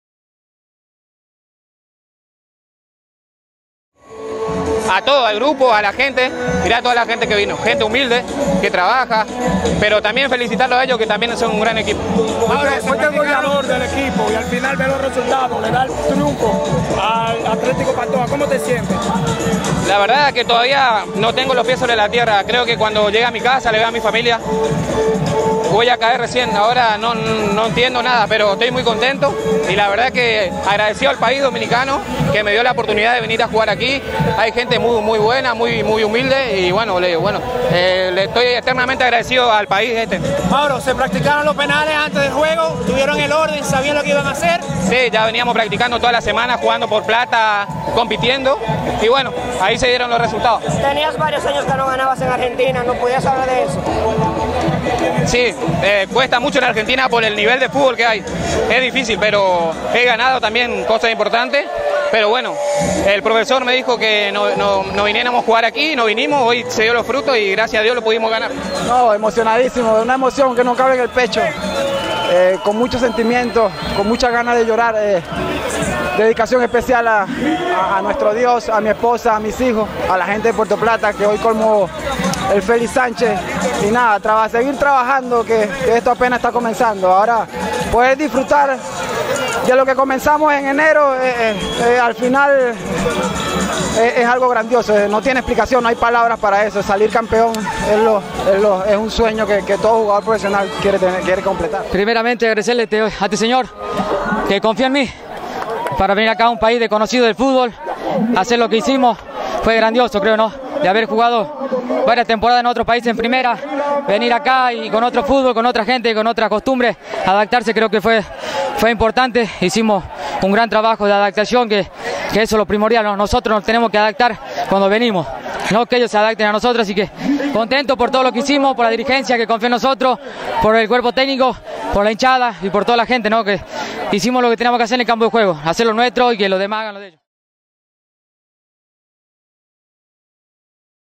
Jugadores del Atlántico FC hablan luego del campeonato
Jugadores del Atlántico FC hablan luego del campeonato obtenido frente al Atlético Pantoja.